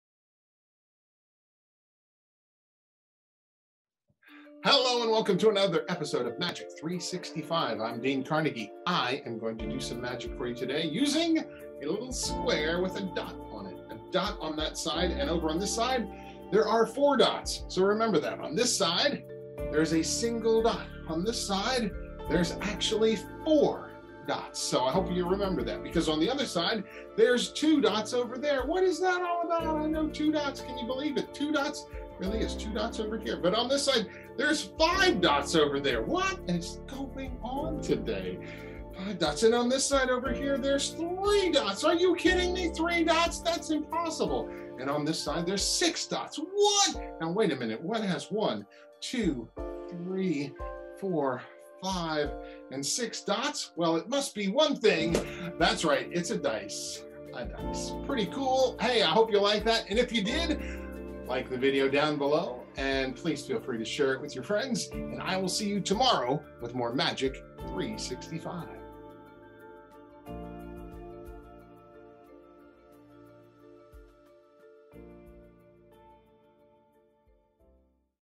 Crazy Spot Trick Magic365
Here is an unusual piece of magic with some spots on a square. They keep changing and then. THEY REALLY CHANGE!!!